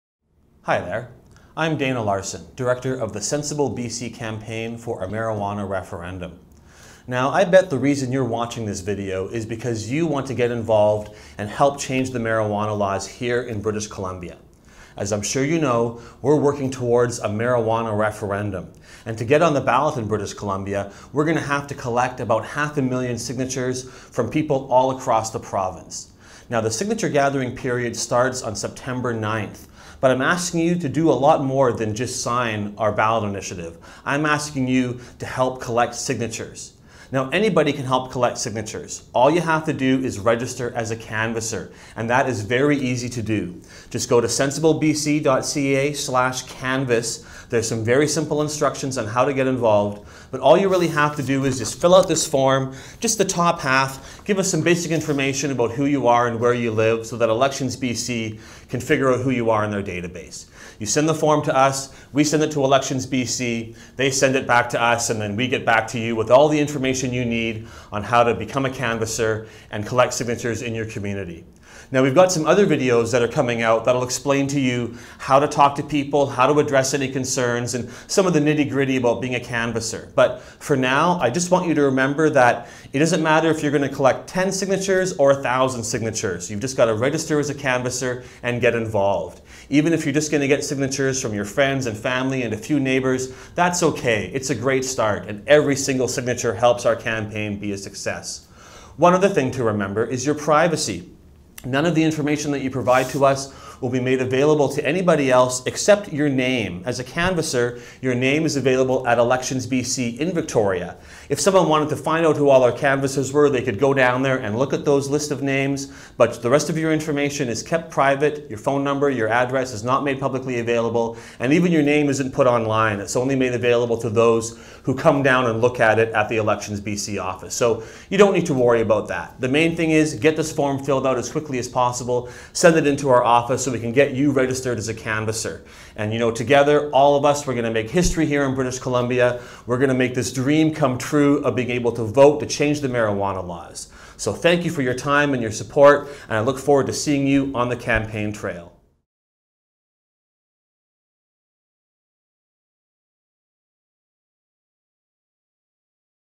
Become a Canvasser for Sensible BC
Register now as a canvasser so you can help collect signatures for Sensible BC this September. It's easy to register, so do it now!